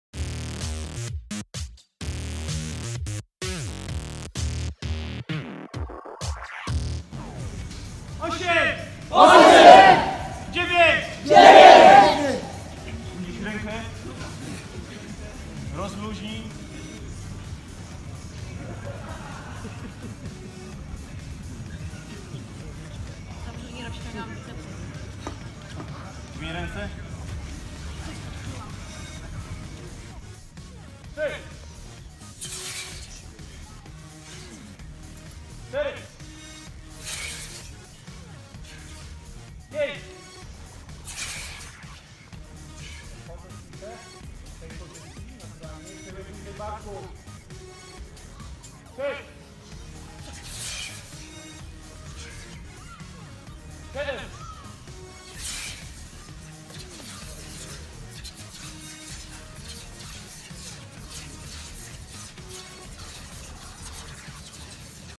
V Ogólnopolski Obóz HardStyle Kettlebell – Łagów 2017.
Za nami V Ogólnopolski Obóz HardStyle Kettlebell – Łagów 2017. Między 19 a 23 lipca 2017 roku uczestnicy obozu nauczyli się poprawnego wykonywania ćwiczeni w oparciu o metodę Hard Style, dowiedzieli się czym są "drabiny" oraz mogli zapoznać się z prawidłowym ustaleniem planu treningowego.  Więcej w tym temacie poczytacie na portalu wzielonej.pl